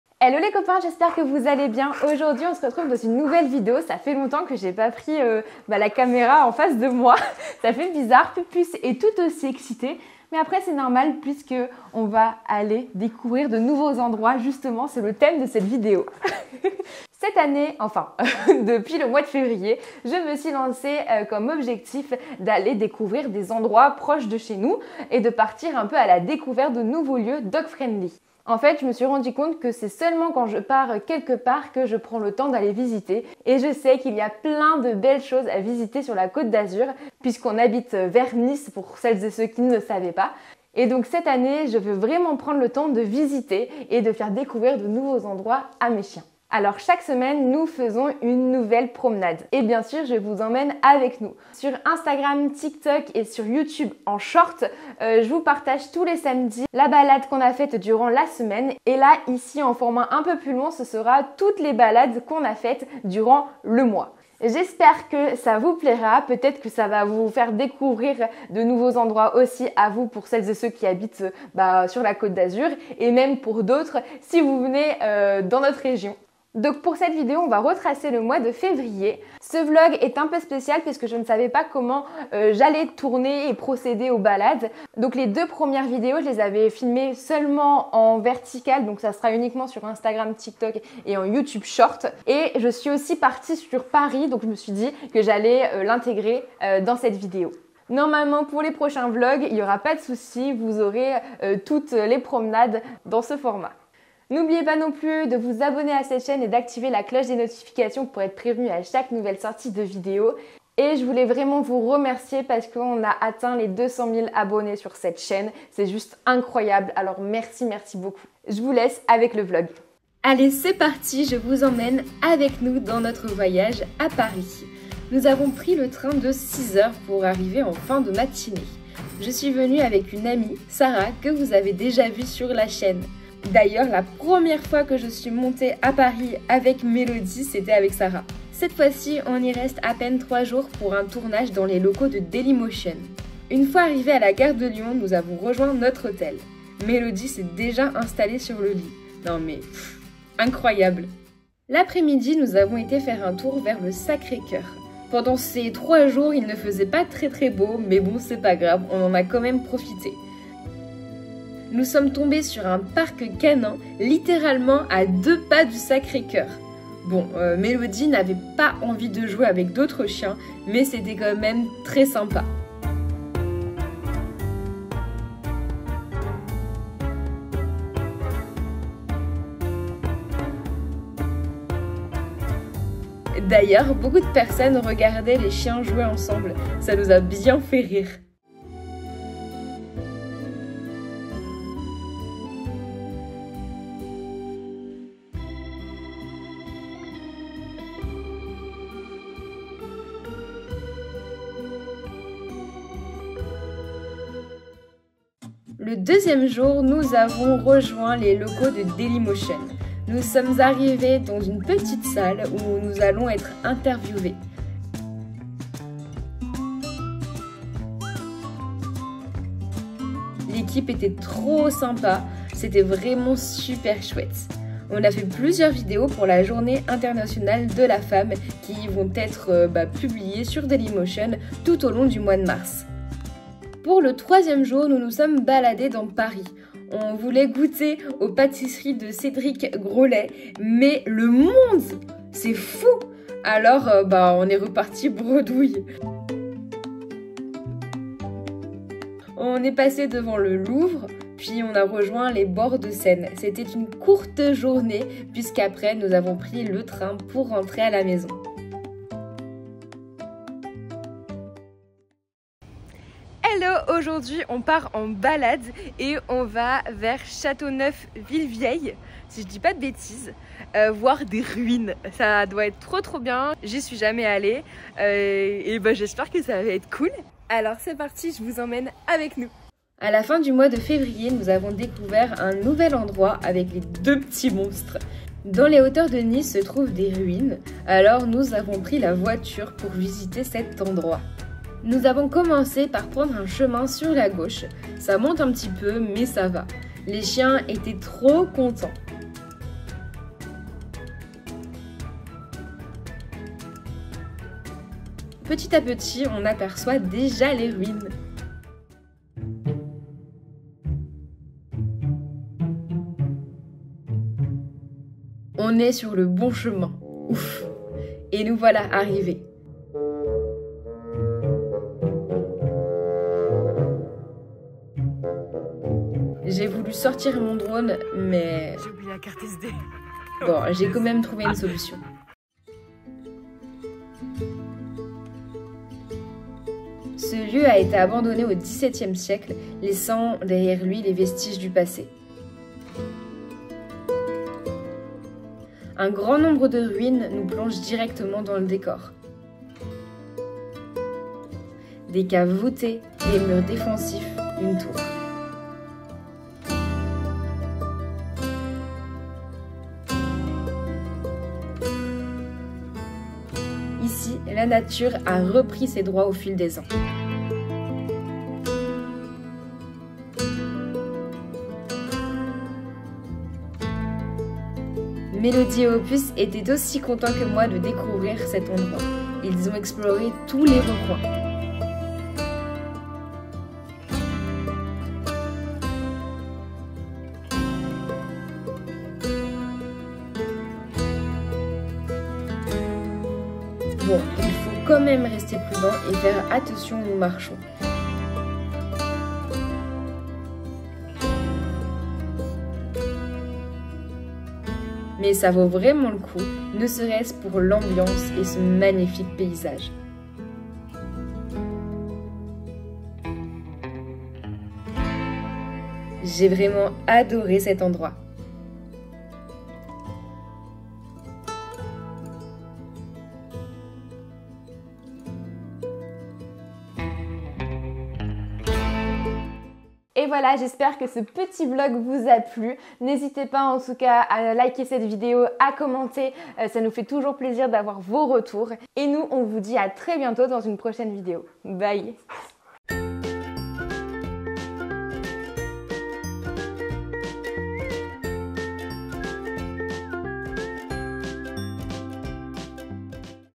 DE PARIS AUX RUINES, DÉCOUVREZ NOTRE MOIS DE FÉVRIER !
Hello les copains, aujourd'hui on se retrouve dans le vlog du mois de février ! Durant ce mois, nous sommes parties à Paris pour un tournage dans les locaux de Dailymotion et nous avons découvert de nouveaux endroits, notamment de magnifiques ruines !

♡ N'hésite pas à me dire ce que tu as pensé de cette vidéo, si elle t'a plu ou non !